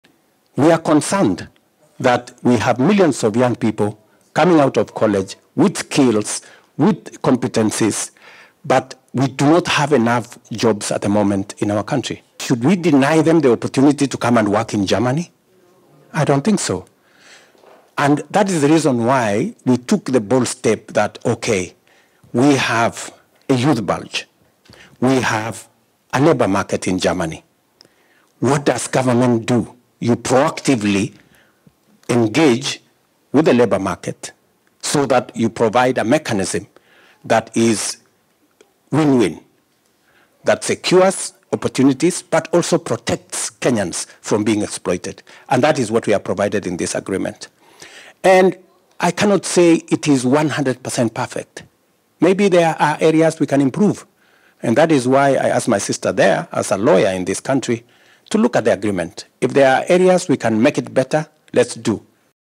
We have proactively engaged the labour market in Germany to ensure the agreement is a win win - Ruto
President William Ruto during the Diaspora Town Hall with Kenyans living in Germany, Berlin, 14th September 2024